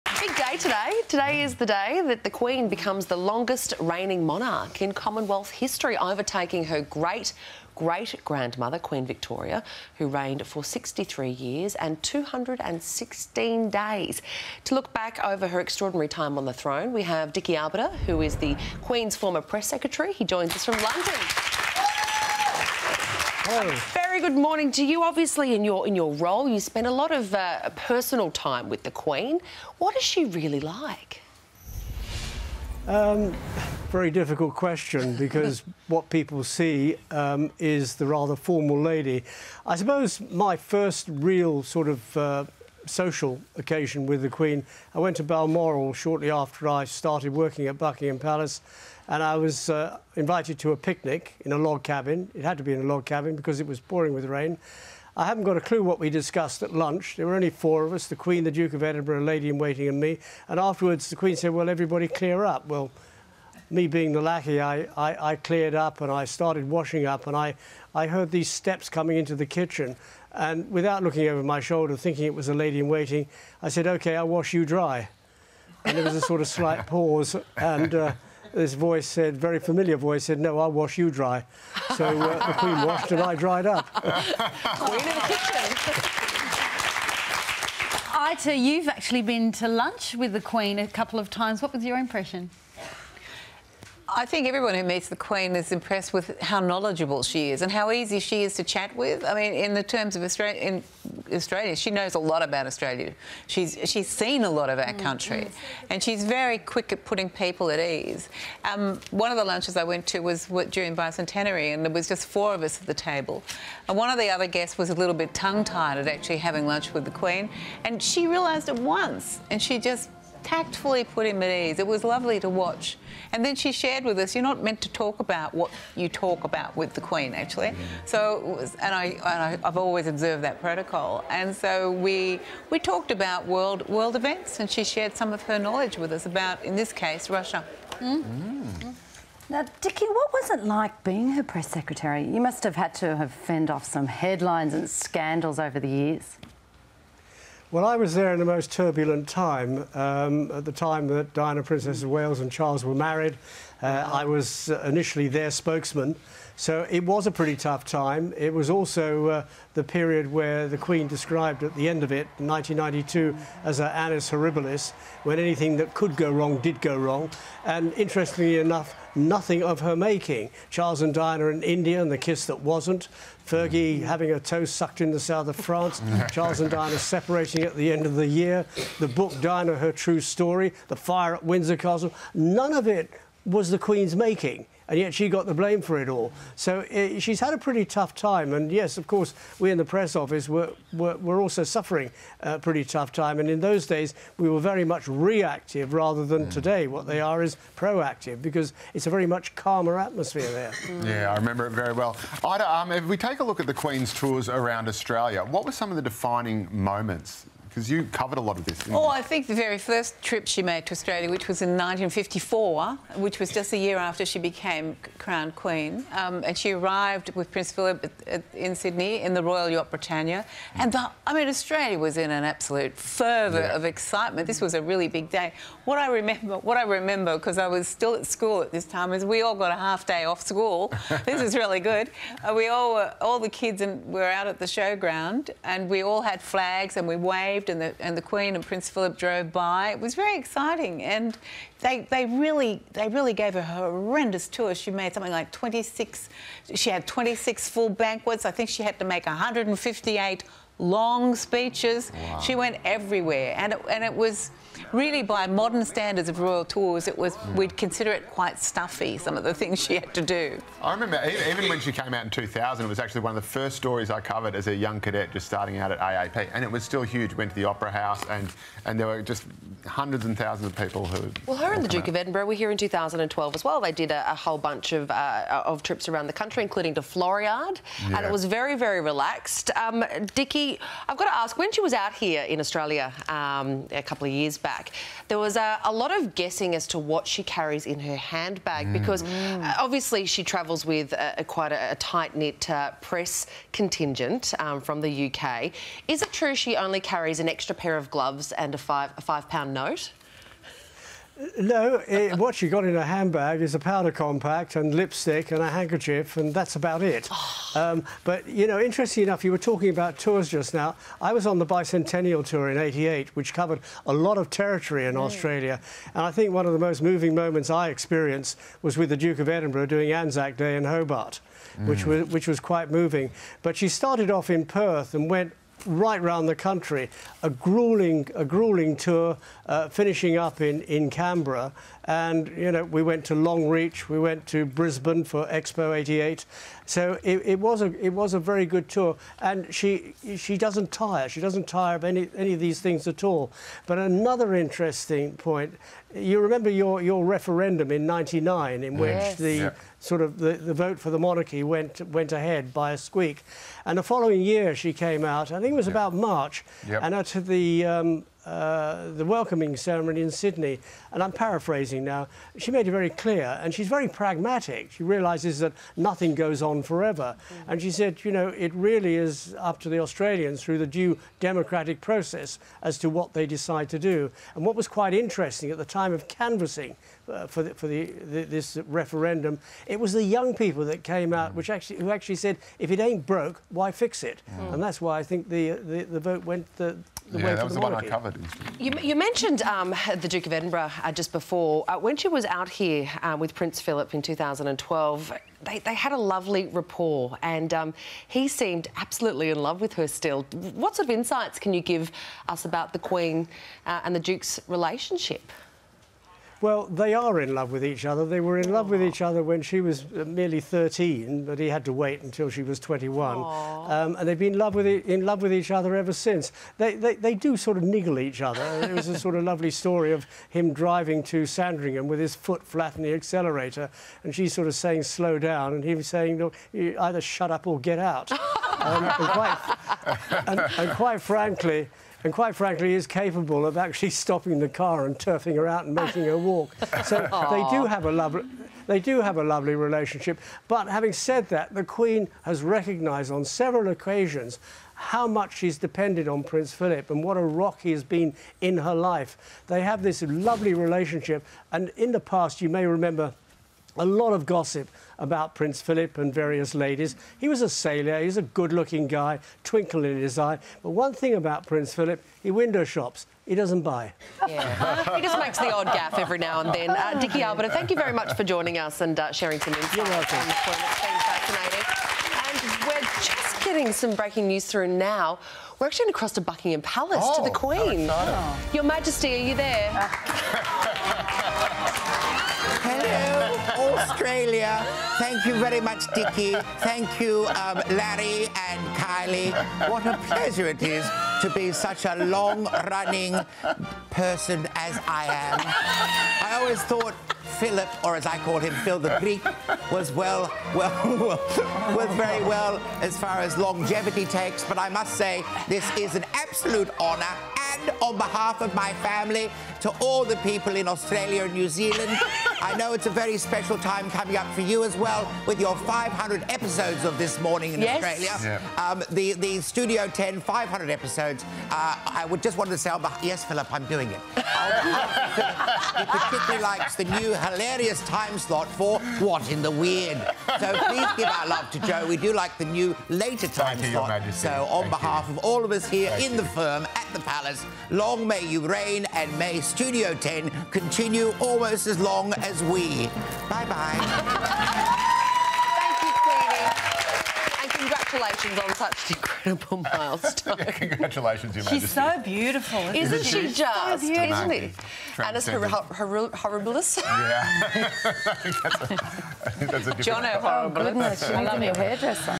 Record Breaking Queen
Queen Elizabeth II becomes the longest serving British monarch - Dickie Arbiter is here to discuss the landmark.